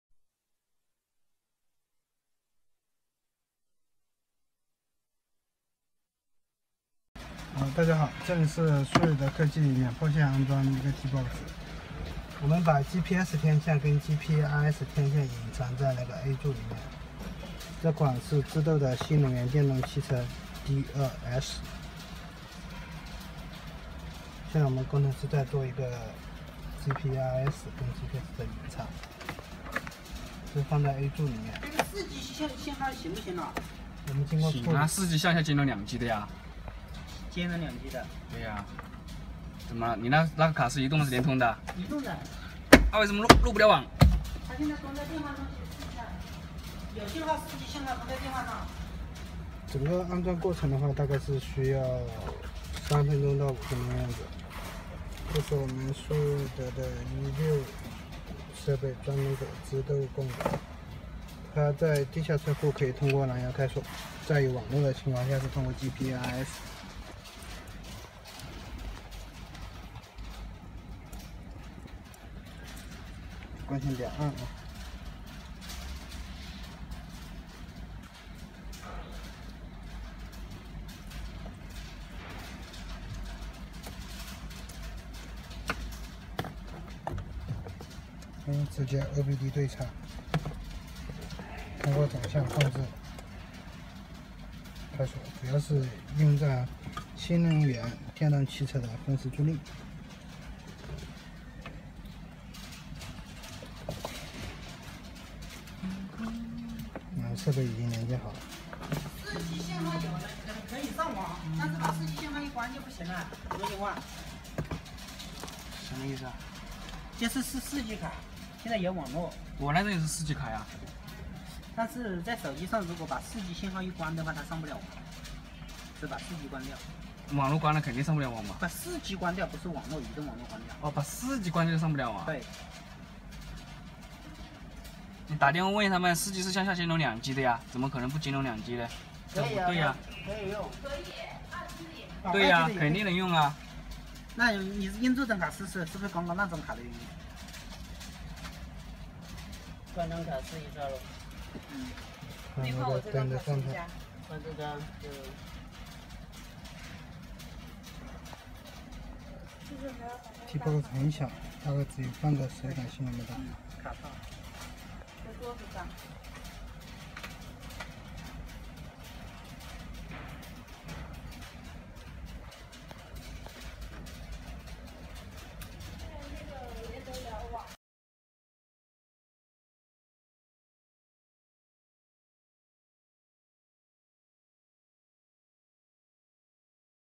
THREAD E6 new energy sharing electric car knowledge beans car TBOX system time - sharing lease
THREAD E6 new energy sharing electric car knowledge beans car TBOX system time - sharing lease:E6 as an electric vehicle and Internet connection unit, THREAD E6 is the electric vehicle CANBUS bus data application core. We will be electric car CAN bus BCM, BMS, DBC data translated into decimal data, through the 4G cellular network transmission to the OEMs and service operations platform database, opened the field of automotive data opportunities for consumers and businesses can provide safe, interesting, Comprehensive driving experience, collaborative OEMs and operating platform companies, found in the new energy vehicle CANBUS data on the smart key car start system, wheel speed calculation TPMS technology, speed steering lights ADAS applications, HUD speed speed display, remote control car and security , Intelligent BMS data upload, full LCD instrument data acquisition display, intelligent headlamp control and speed artificial intelligence, shared car time-sharing leasing, unmanned steering data acquisition and perception and other areas of business opportunities and scene applications. Through the E6 box to collect the total mileage of electric vehicles, the remaining power, mileage, the overall voltage, current, charging status, remote door and control the car to start and collect GPS location information. Widely used in car sharing travel, intelligent transportation, Intelligent, fieldbus control.